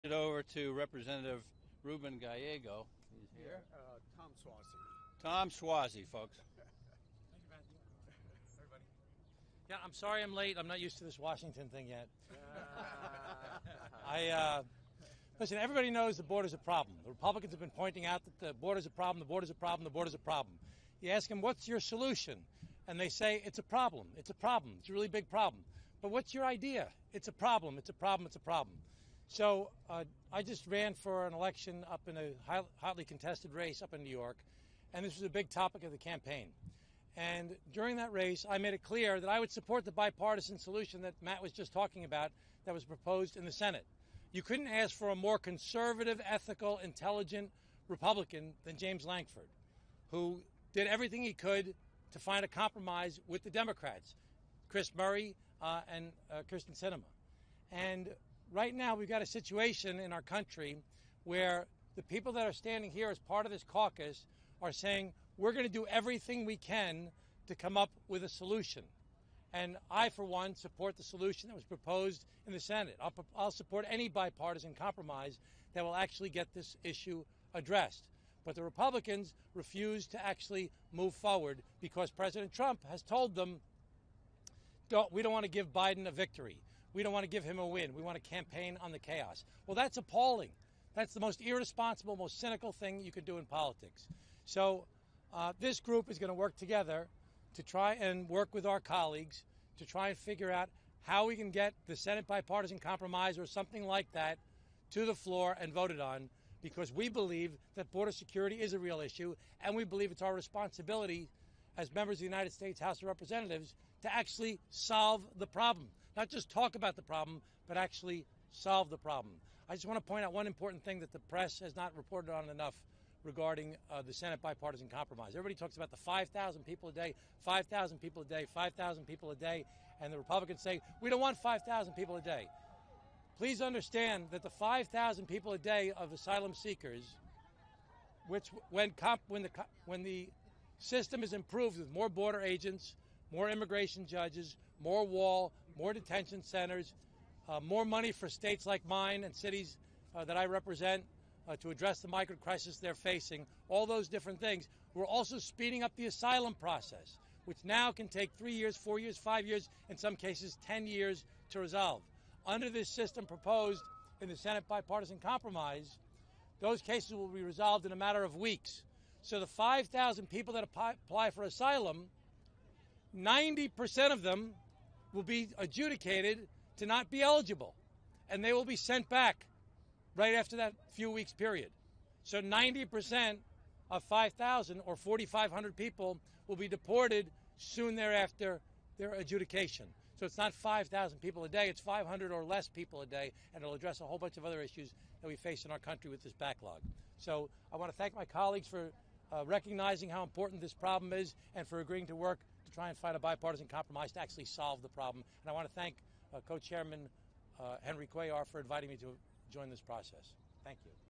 ‘Most Cynical Thing’: Tom Suozzi Rips House GOP For Not Taking On Action On The Border To Help Trump
During a press conference on Tuesday, Rep. Tom Suozzi (D-NY) spoke about the creation of a House Democratic task force on the southern border.